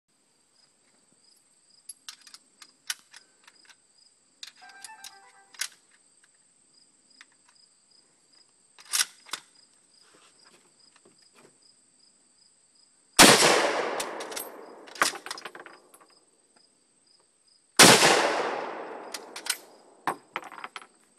WWII Arisaka Type 99
My friend Evan shooting my Arisaka Type 99 Last Ditch Rifle made in 1945.